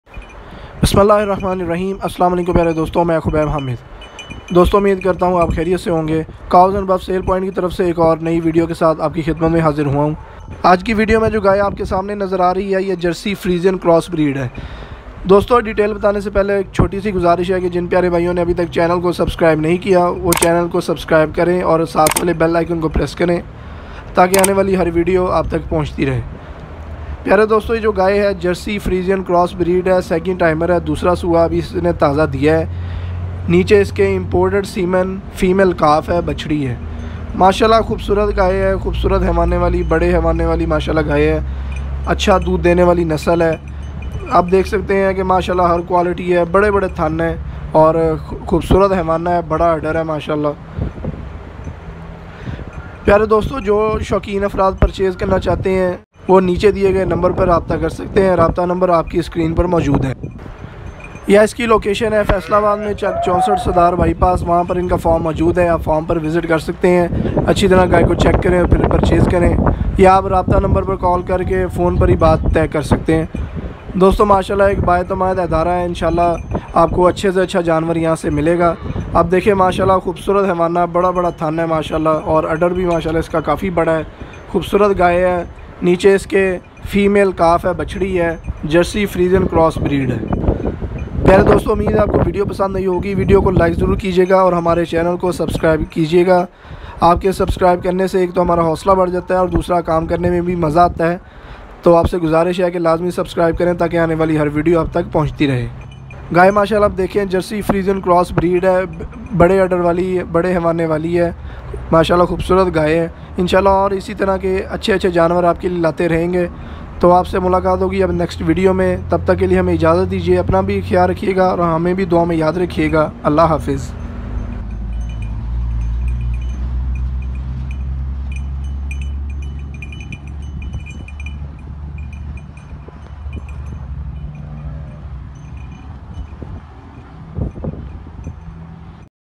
2nd Lactation Cow for Sale | Jersey cow in Faisalabad
Jersey friesian Cow for sale in Faisalabad.Cow is in 2nd Lactation with imported semen Female Calf.Its expected yield of milk is 25-30 litres.You can Purchase it by Calling on the number mentioned in the Video.Cow is freshly calved with a huge udder.Thanks for watching and be a part of Cows and Buffs Sale Point.